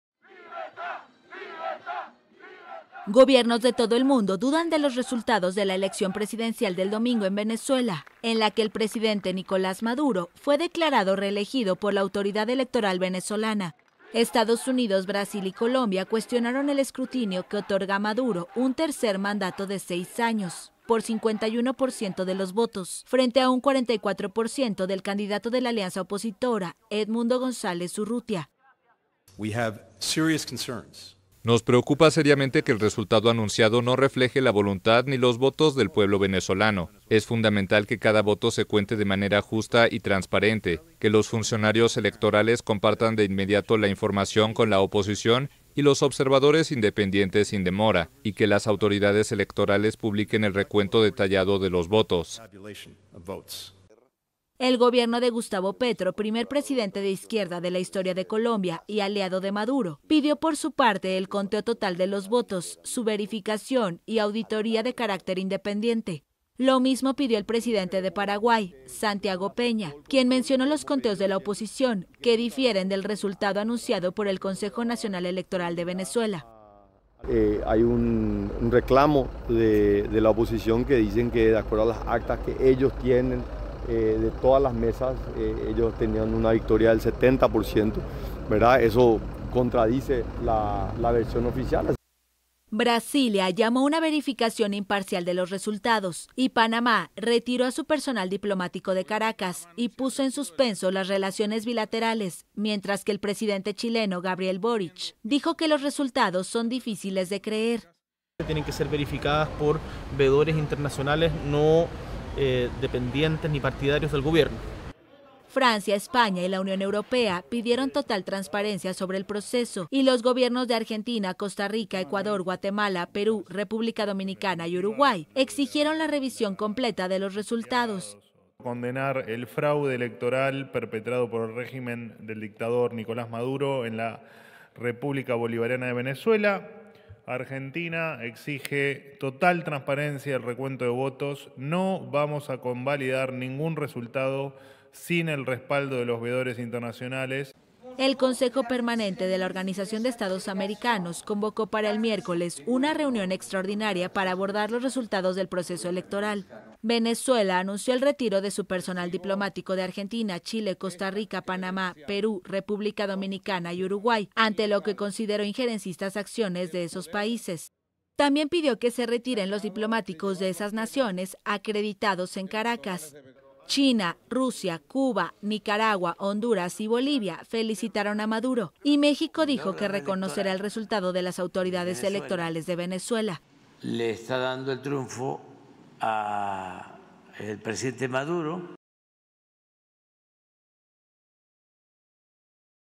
Gobiernos del mundo cuestionan resultados que dan triunfo a Maduro en Venezuela | AFP
Gobiernos de todo el mundo dudan de los resultados de la elección presidencial del domingo en Venezuela en el que el presidente Nicolás Maduro fue declarado reelegido por la autoridad electoral venezolana.